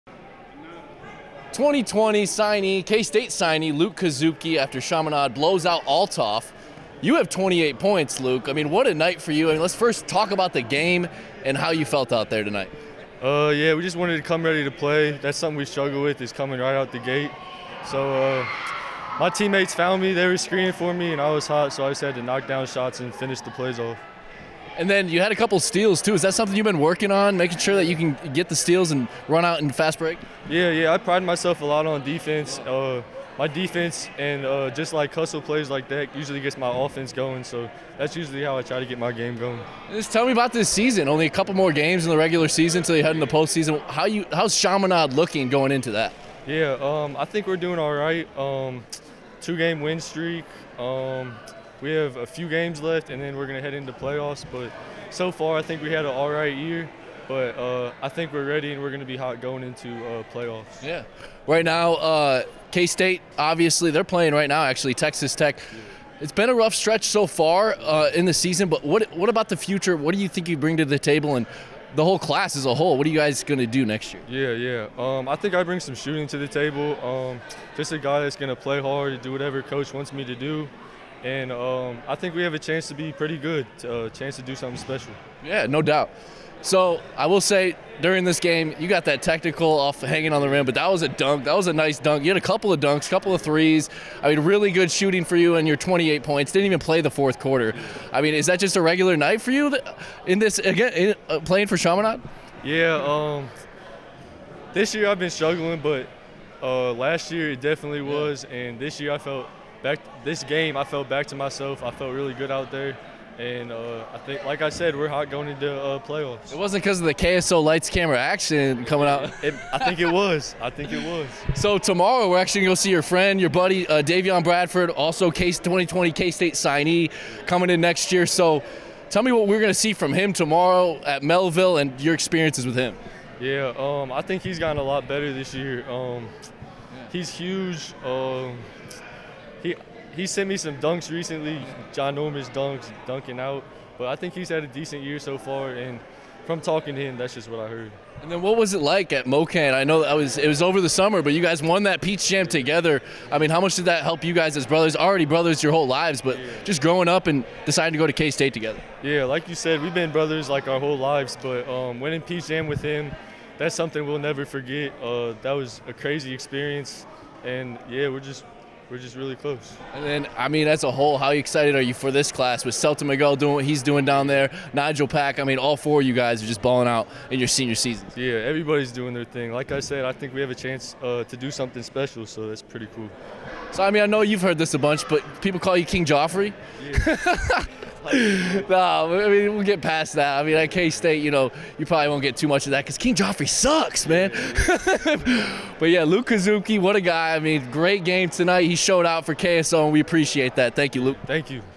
K-State 2020 Signee Luke Kasubke after 28 Points in 3 Quarters
Hear from Chaminade's Luke Kasubke after he puts up 28 points in the first three quarters in the blowout win over Altoff Catholic High.